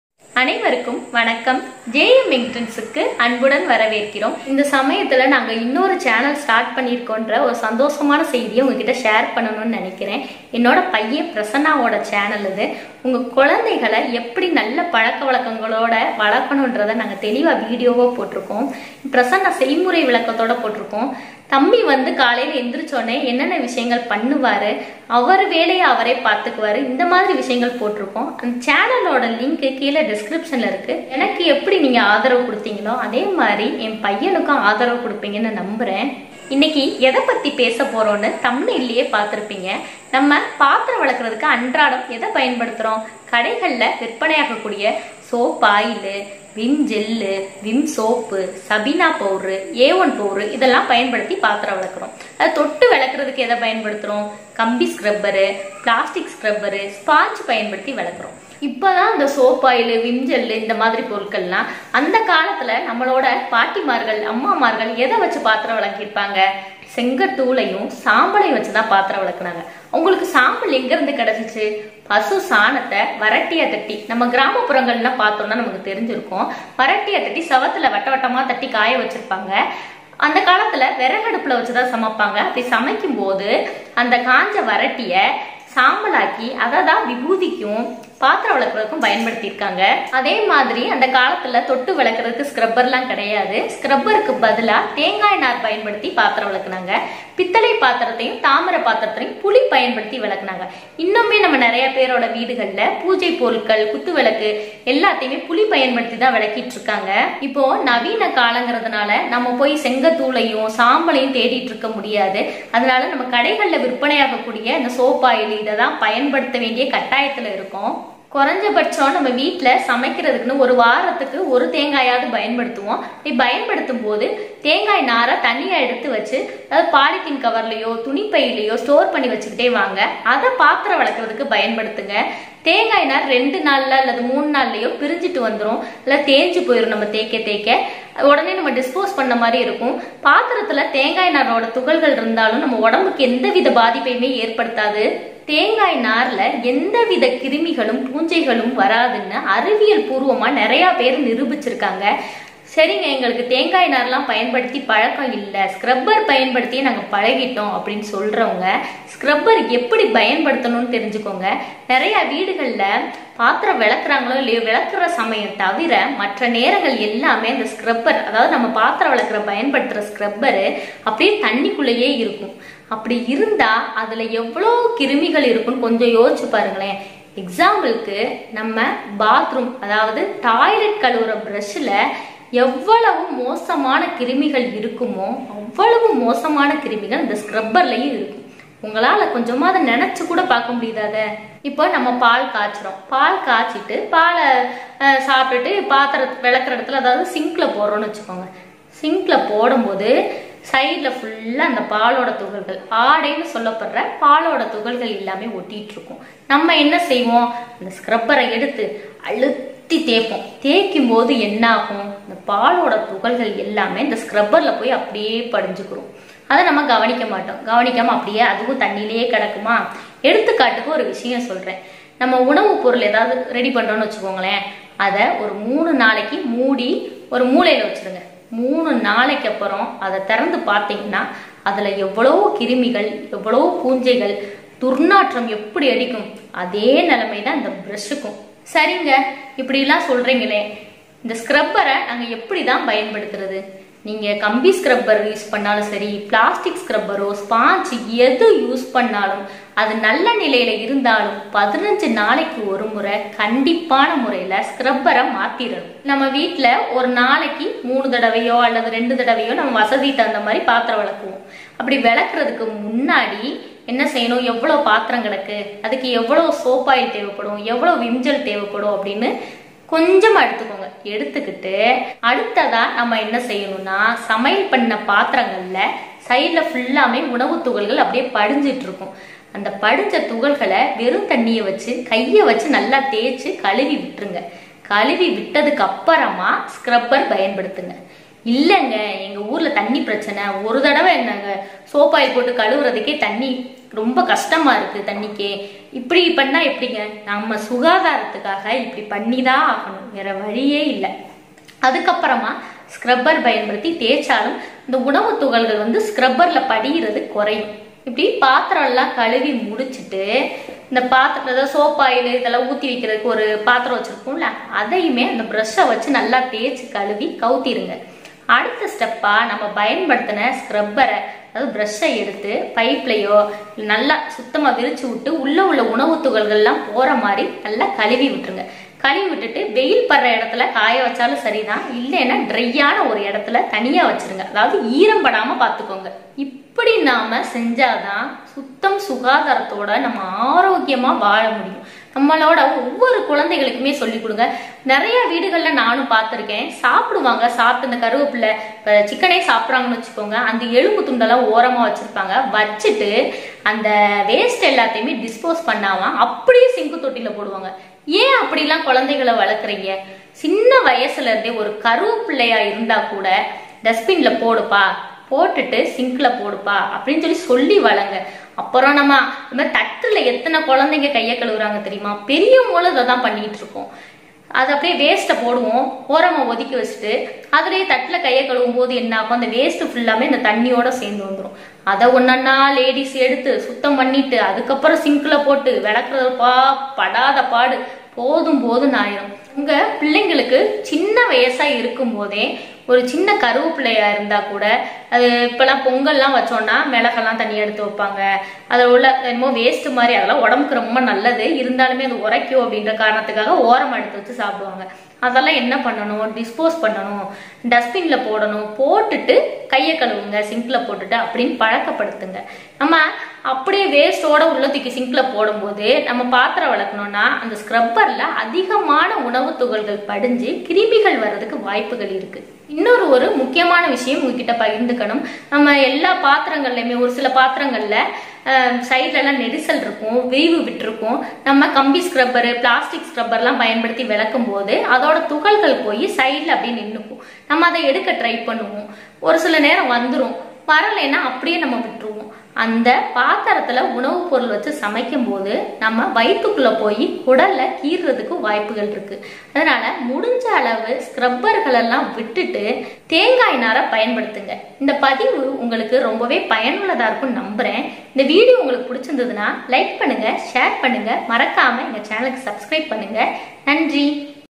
Process of washing dishes by hand in tamil/Defects of dish scrubber brush in tamil/Dishwashing scrub
Dishwashing scrubber is used to clean the cooking vessels.Dishwashing scrubber should contains many models 1.plastic scrubber 2.stainless steel scrubber 3.sponge scrubber.Washing is the process of after cooking.Dish scrubber brush should contains many defects.Coconut fiber is the best scrubber for dishwashing.Stainless steel scrubber parts will affecting the inner part of the body.Dishwashing scrubber will produce the lot of germs and bacteria.Lot of dishwashing liquid and dishwashing products are available in markets.Coconut fiber dishwashing scrubber is used to protect from germs.